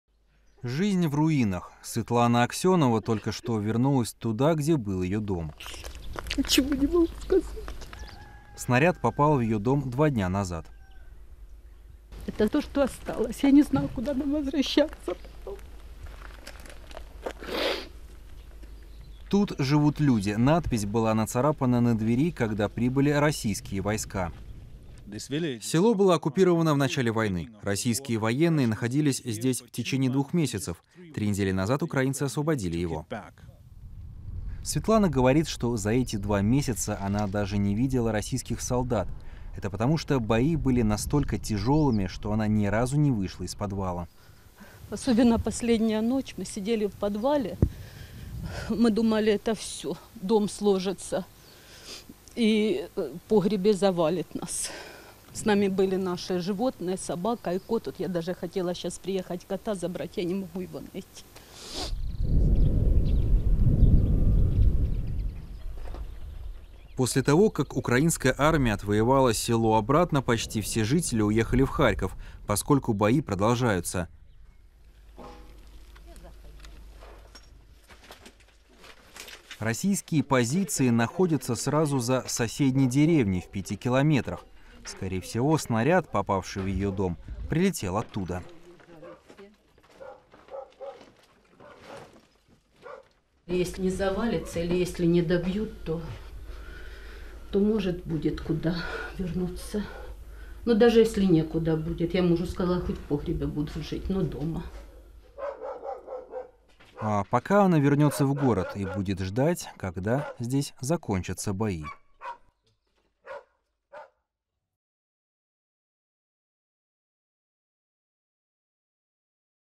Что осталось от украинского села Русская Лозовая под Харьковом
Корреспондент DW Матиас Бёлингер побывал в Русской Лозовой, украинском селе под Харьковом, откуда недавно были вытеснены ВС РФ.